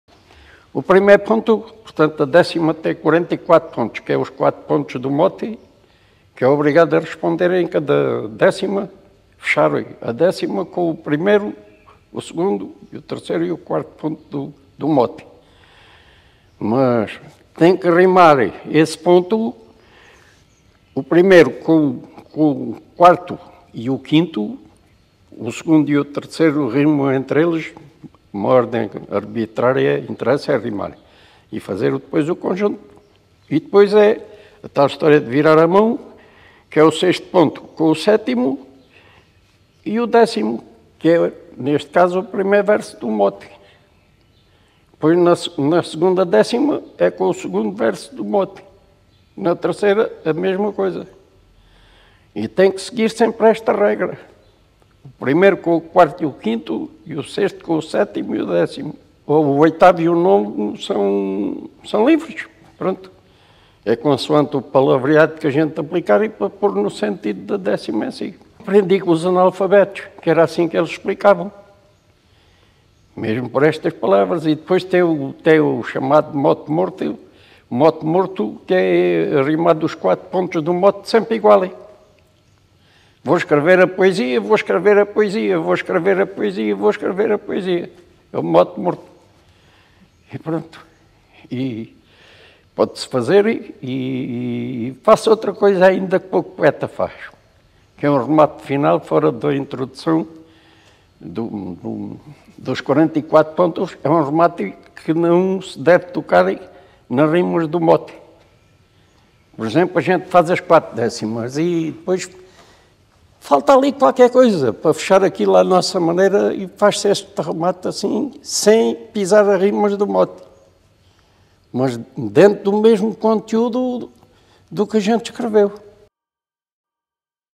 Poeta Silvais - A construção das décimas
Poeta Silvais
(Manuel Joaquim Carvalhal)
Poeta Alentejano
Évora
Recolha realizada no âmbito do curso "Registo de Património cultural imaterial" com a participação dos formandos.
Turma de 15 e 16 de abril, Évora.